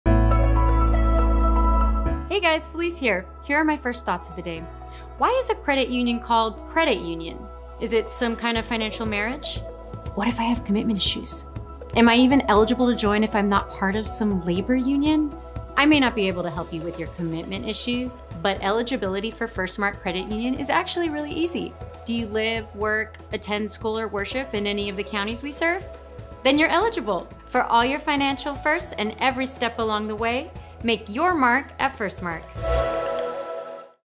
Feliz's First Thoughts | Am I Eligible?
It's not hard to join a credit union. Learn how you can be a part of Firstmark Credit Union.